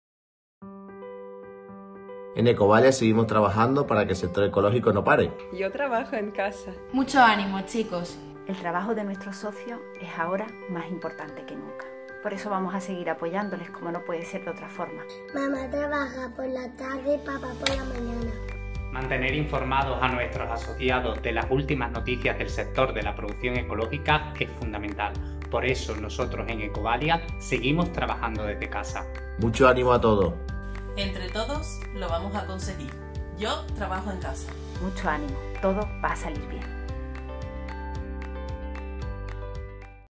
Ecovalia continúa con su actividad
La asociación profesional española referente en producción ecológica continúa con su actividad trabajando desde casa.